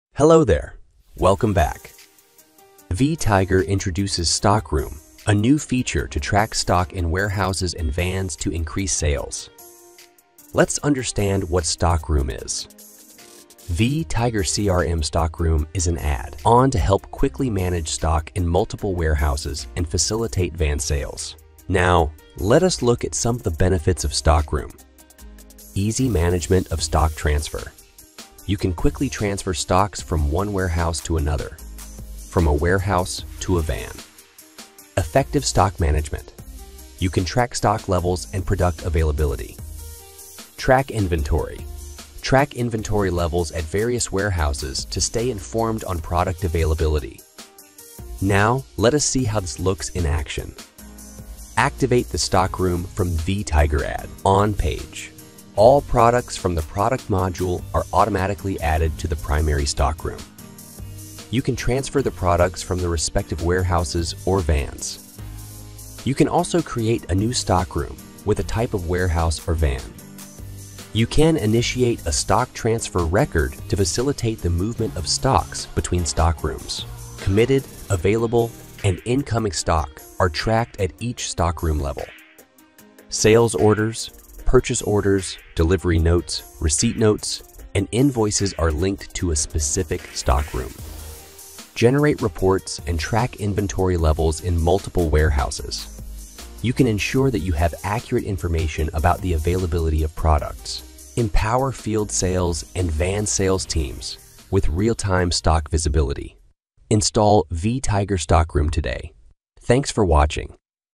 Vtiger Stockroom
In this video, we will walk you through the overview of Vtiger Stockroom.